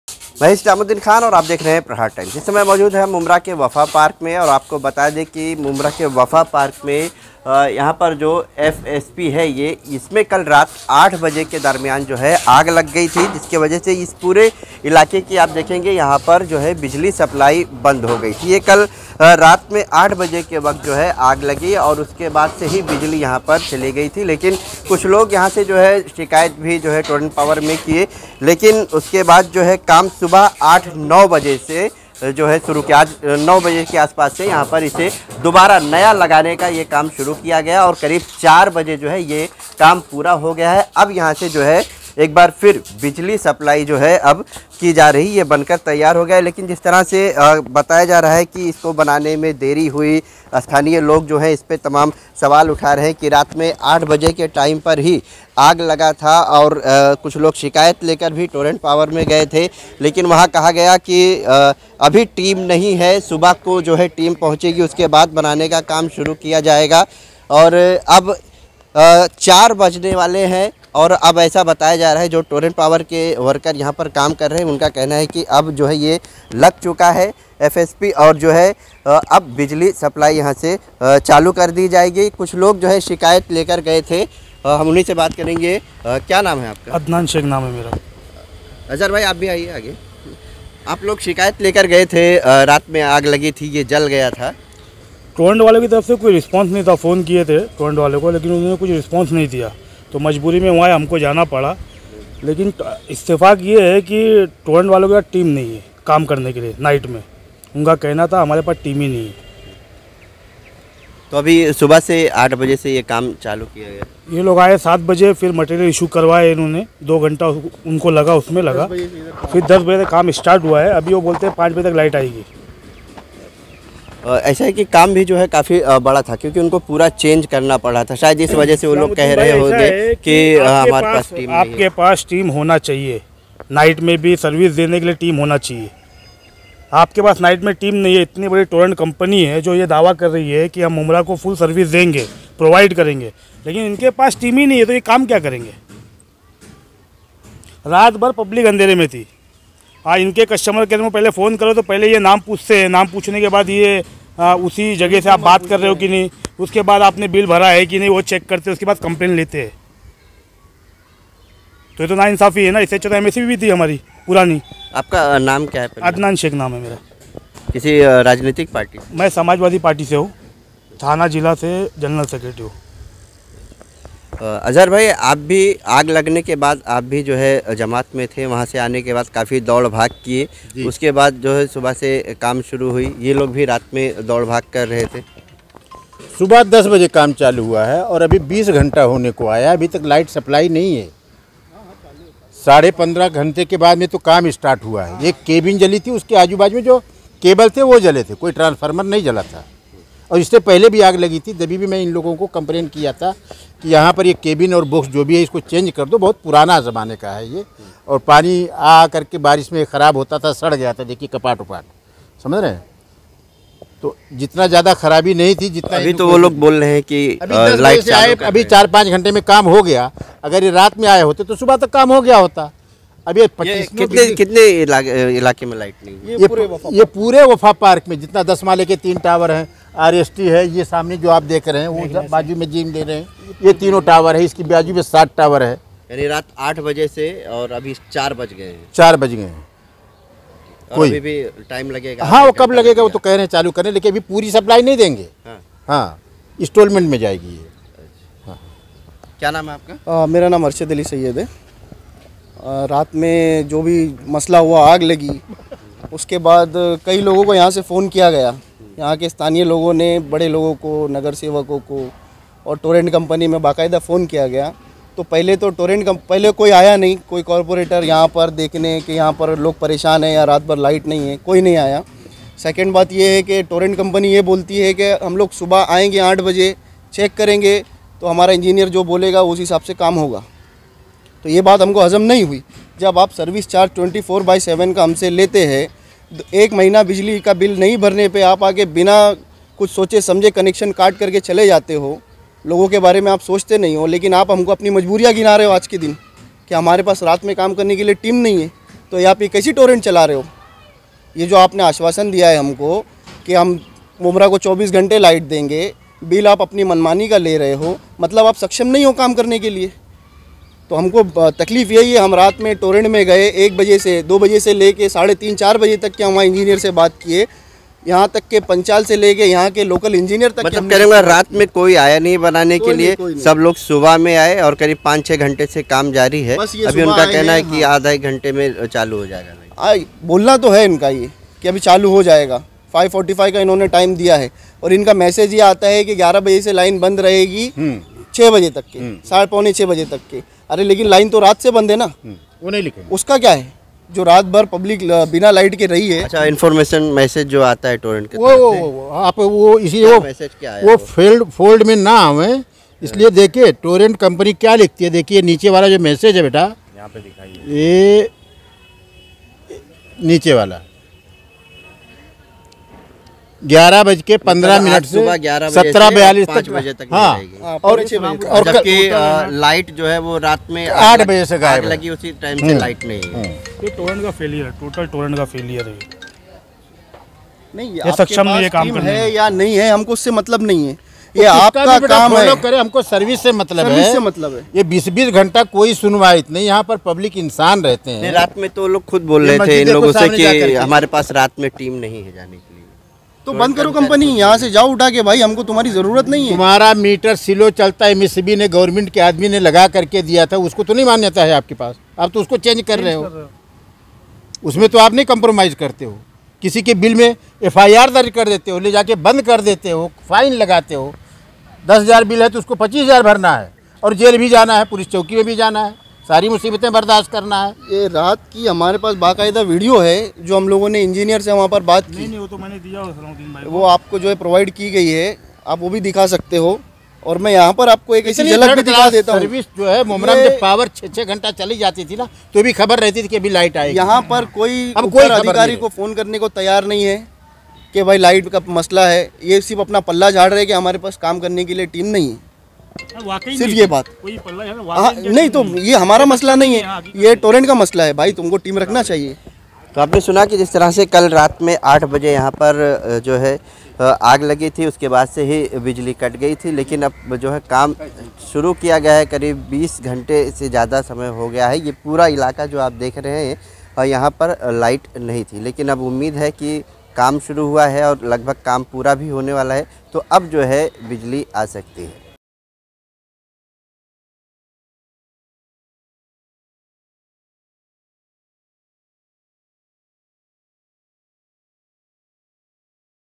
टोरेंट पावर में शिकायत के बाद भी 20 घंटे से बिजली गायब || Islamuddin Khan || Aapka Prahar Times
आग लगने की वजह से 20 घंटे से भी ज्यादा समय से गायब बिजली || Islamuddin Khan